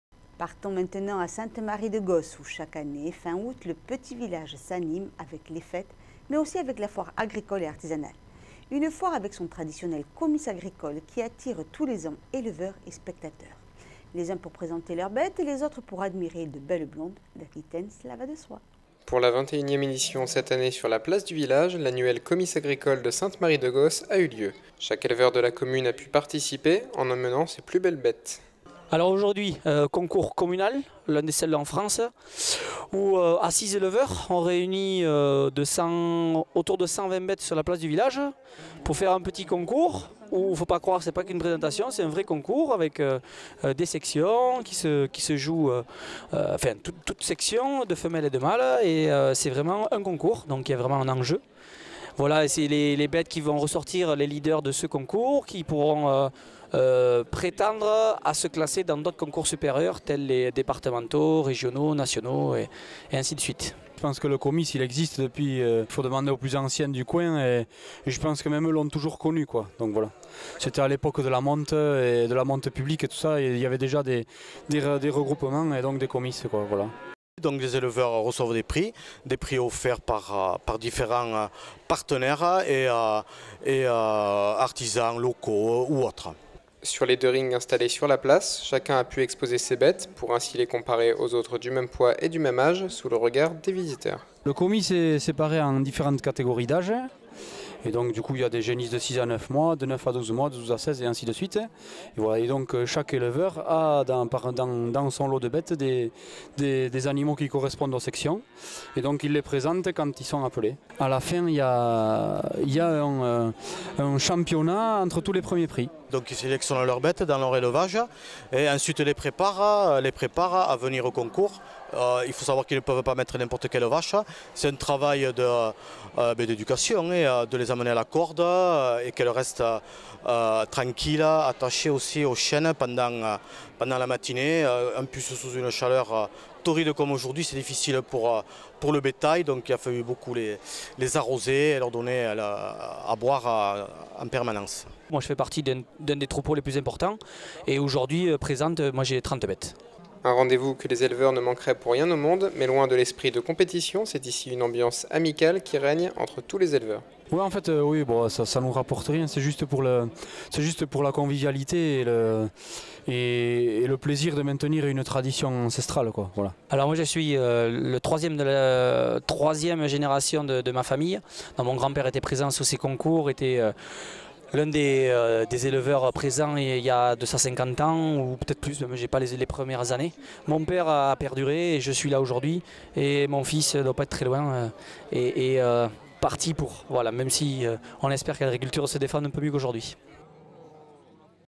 Foire à Sainte Marie de Gosse : le comice agricole
Partons maintenant à Sainte Marie de Gosse où chaque année fin Août, le petit village s’anime avec les fêtes mais aussi la foire agricole et artisanale. Une foire avec son traditionnel comice agricole qui attirent tout les ans éleveurs et spectateurs. Les uns pour présenter leur bêtes, les autres pour admirer de belles blondes (d’Aquitaine cela va de soit….).
MACS / Sainte Marie de Gosse, 2 Septembre 2016